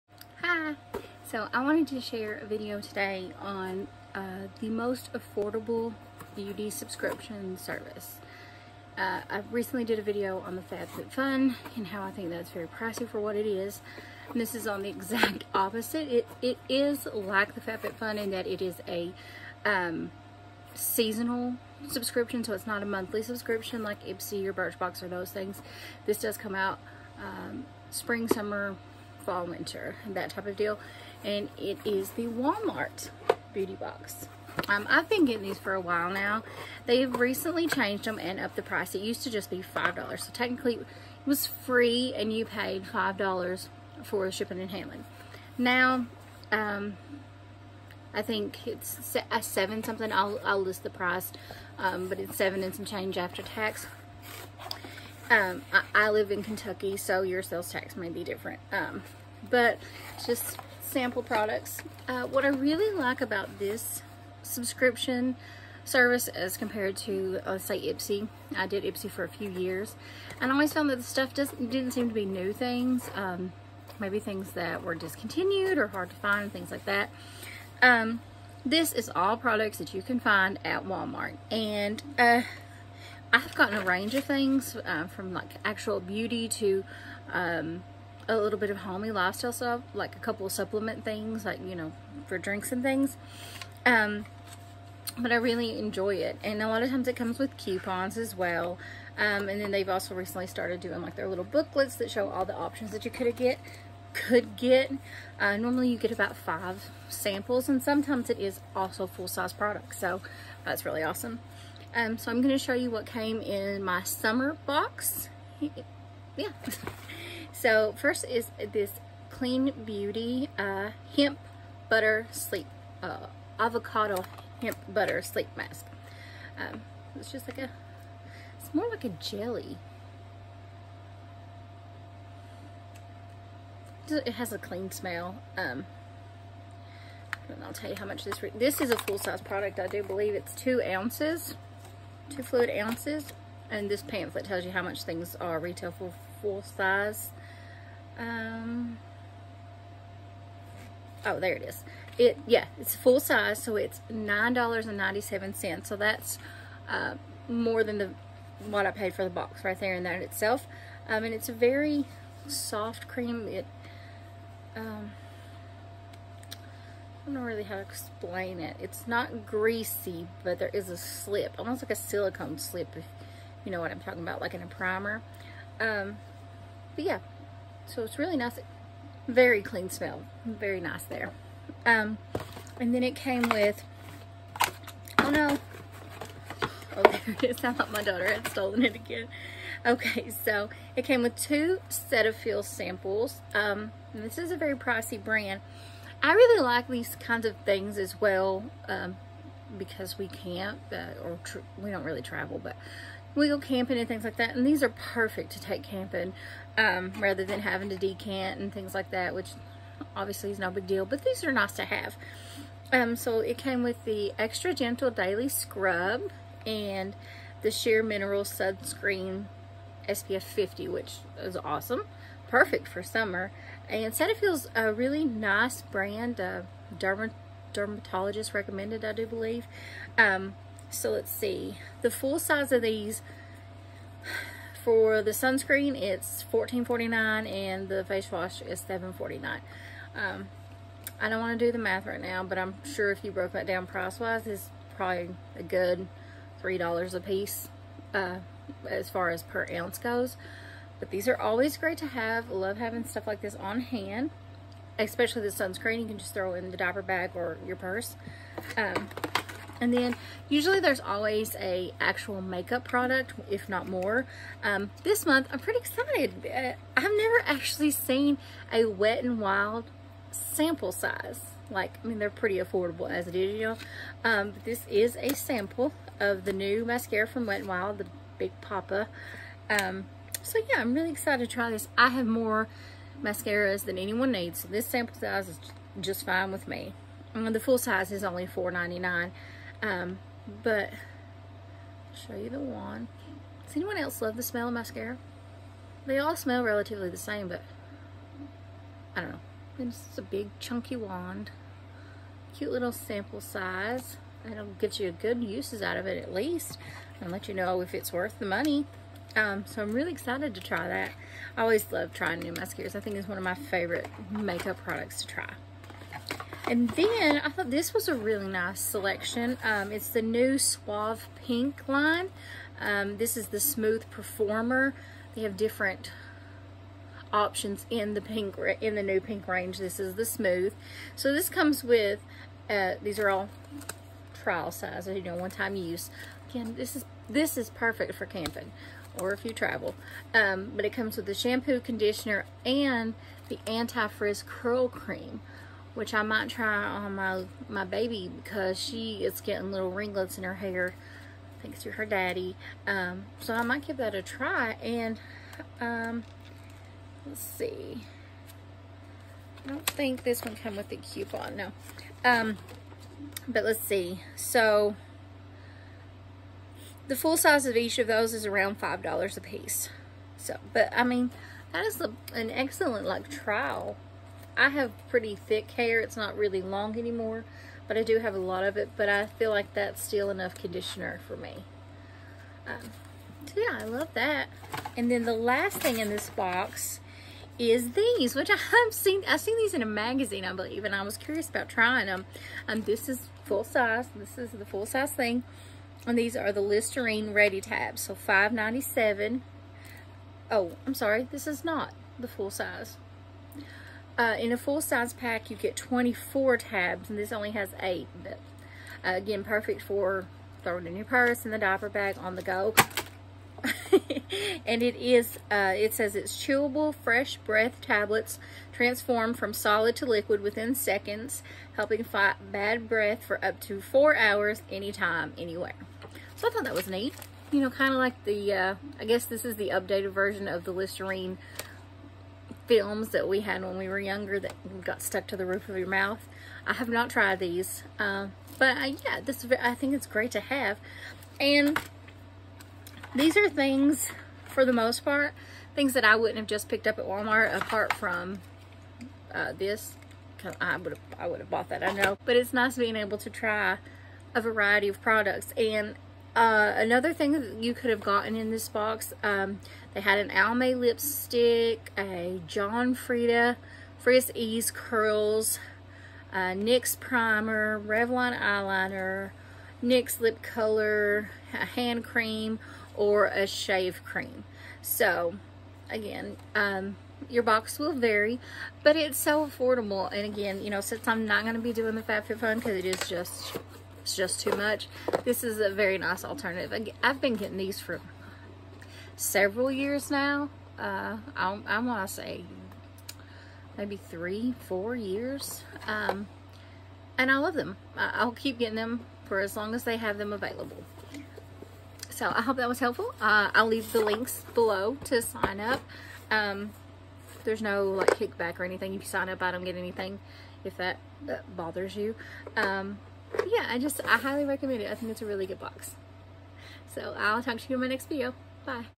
Most Affordable Beauty Subscription Box | Wal*Mart Beauty
Here is my review on the Walmart Beauty Subscription box. What is your favorite subscription box? Are you a sucker for sample/ travel sizes like me?

xx Christine

$6.98 (+tax)